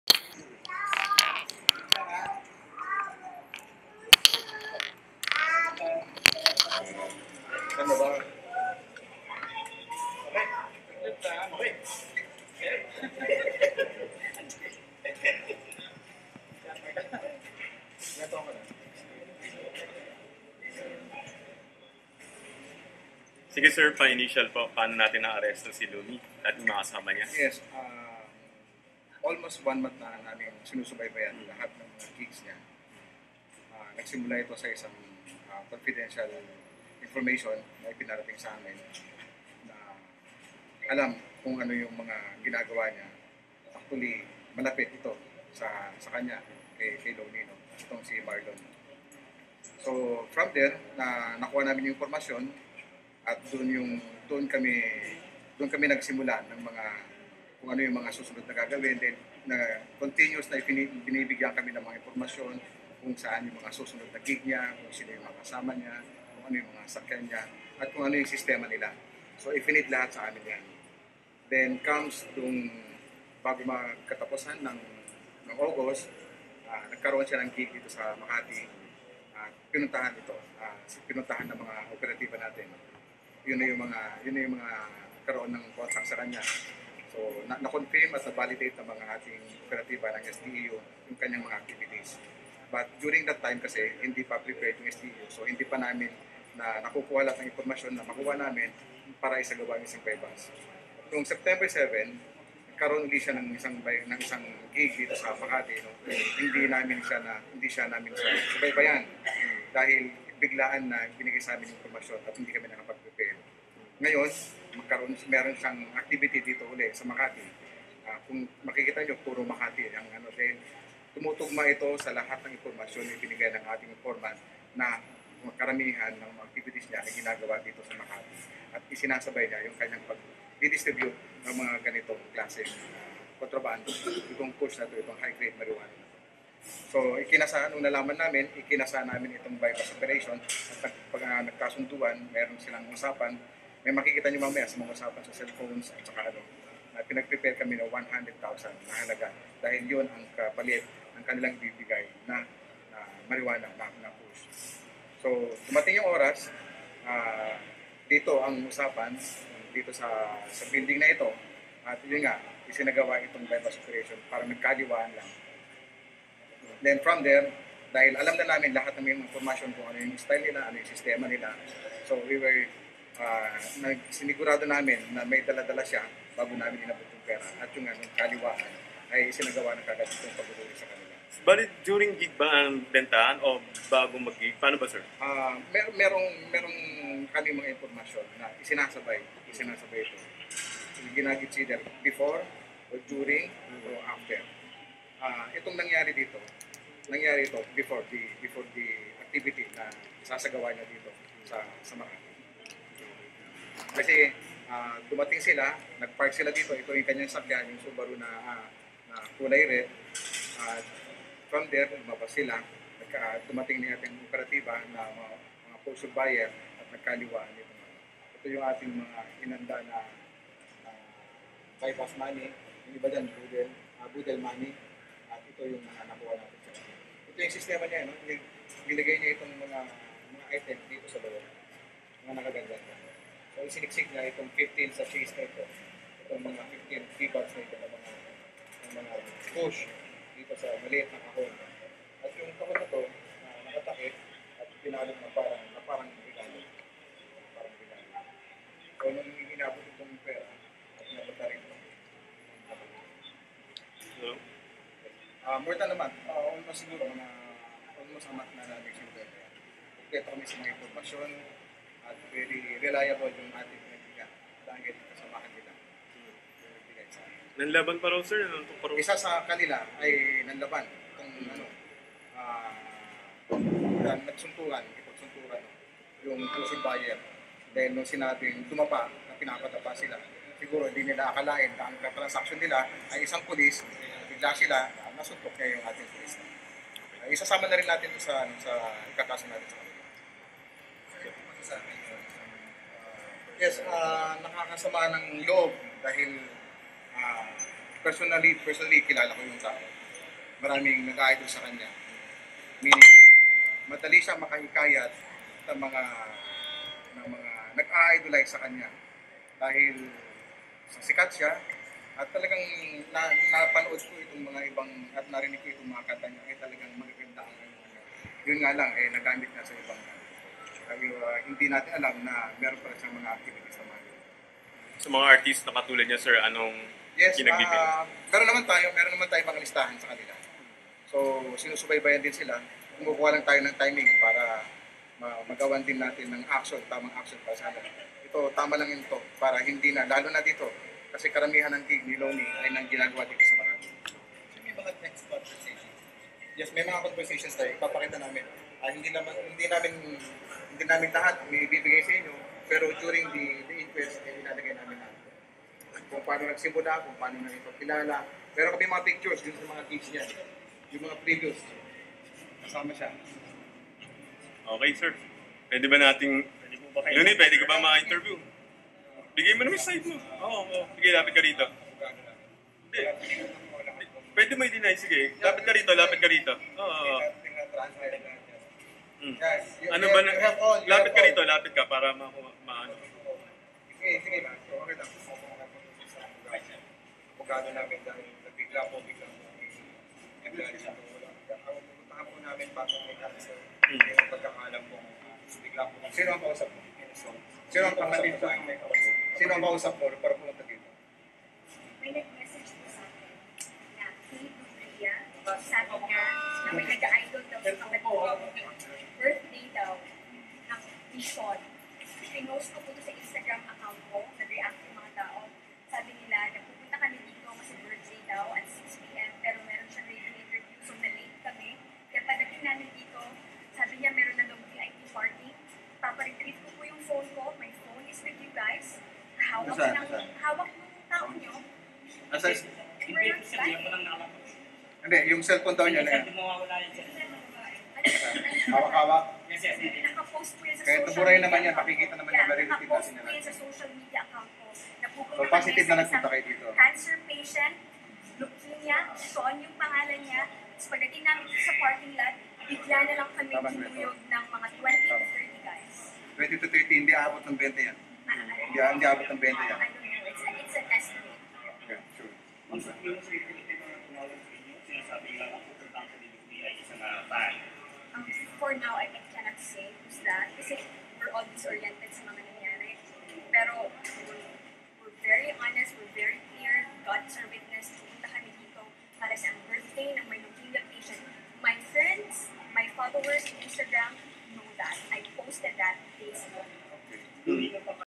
LOONIE ARRESTED
SIKAT NA RAPPER NG FLIPTOP ARESTADO SA BUY BUST OPERATION